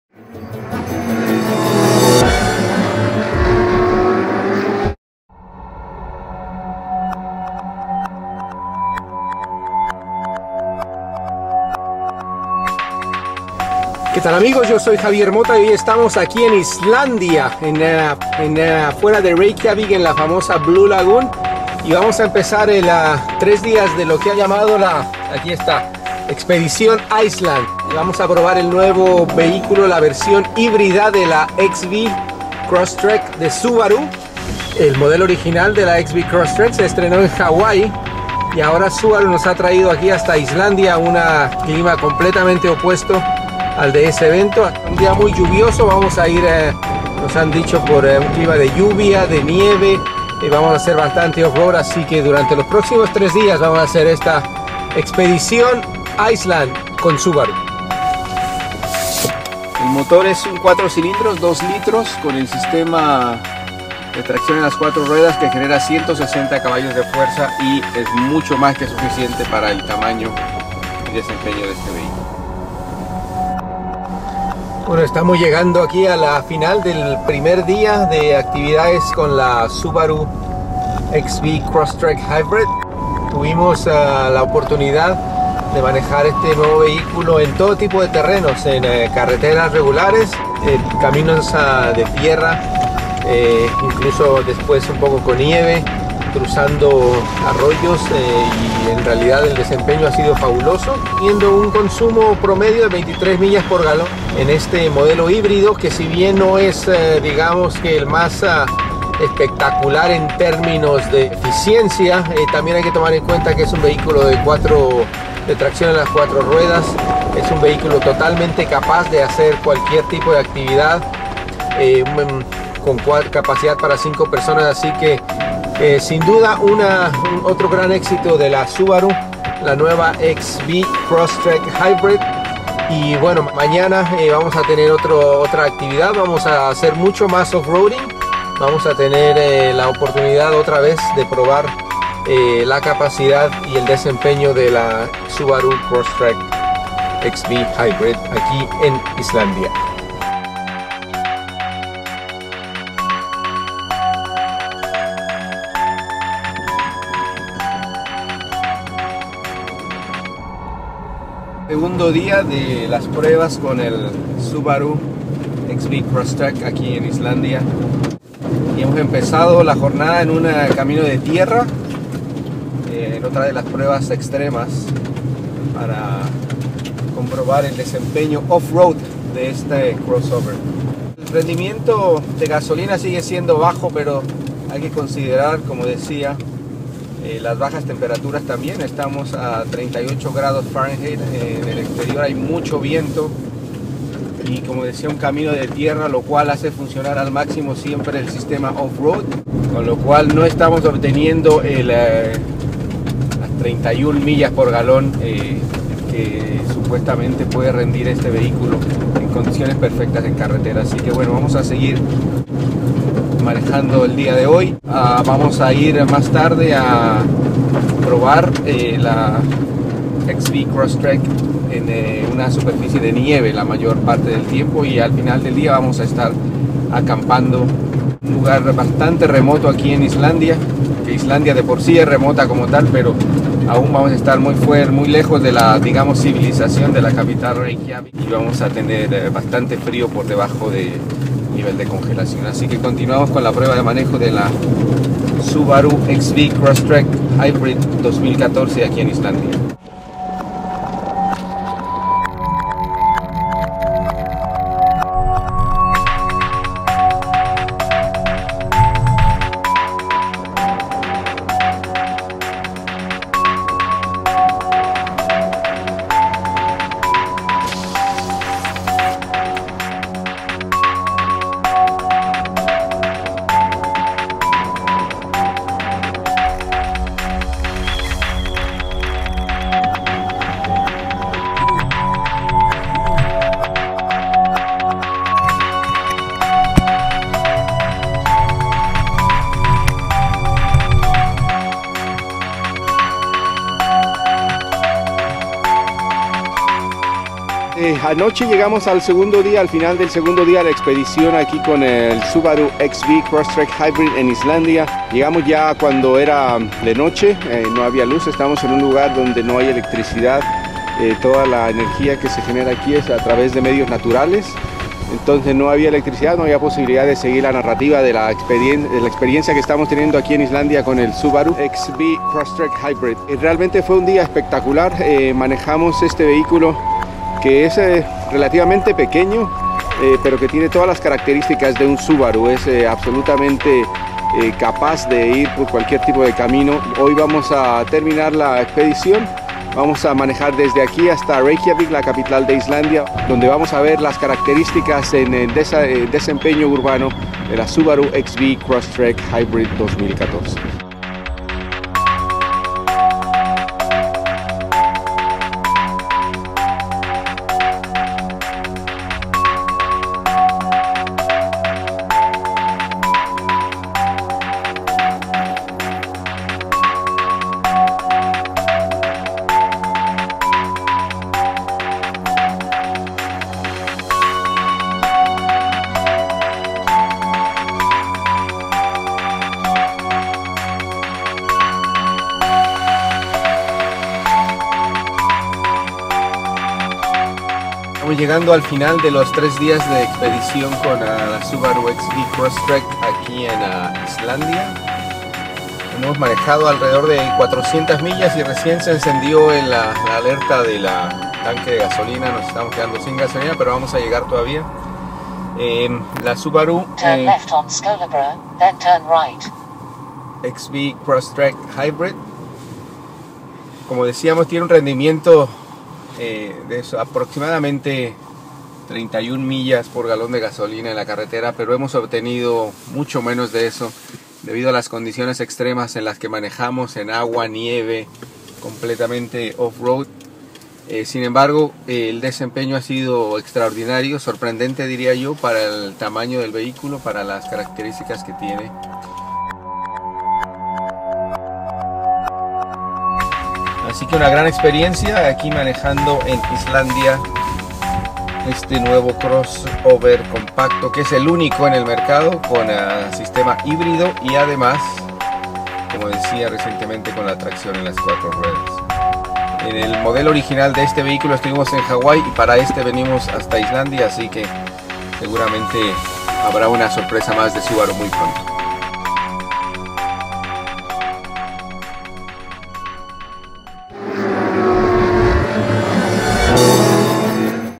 2014 Subaru XV Crosstrek Hybrid - Iceland Expedition - Español
Subaru eligió las montañas y los valles volcánicos de Islandia para la presentación del nuevo crossover XV Crosstrek Hybrid 2014, el primer vehículo híbrido producido por la firma japonesa que saldrá a la venta en Estados Unidos al final del 2013 con un precio inicial de $25,995.

Además de las ventajas de la tecnología híbrida, esta nueva versión del Subaru XV Crosstrek, ofrece todas las características típicas de la marca con extraordinario manejo en condiciones normales o en las más extremas off-road, como los terrenos de grava volcánica, agua, nieve, hielo y tierra, a los que fue sometido en Islandia.